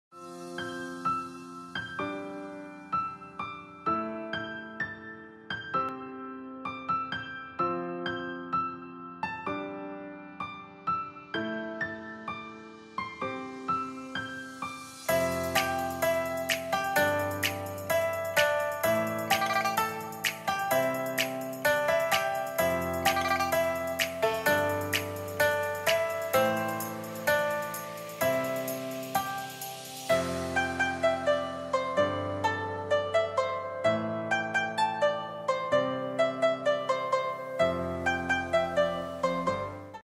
Refrigeration Equipment, Bitzer Condensing Unit
1. The surface of the galvanized sheet shell is sprayed with plastic.
2. Blue fin, there is a layer of anti-corrosion material on the surface of the fin.
3. The copper pipe connection adopts a reducing joint, the welding point is firm, and it is not easy to leak fluorine.
4. Sufficient condenser heat exchange area, good heat dissipation, and good system cooling effect.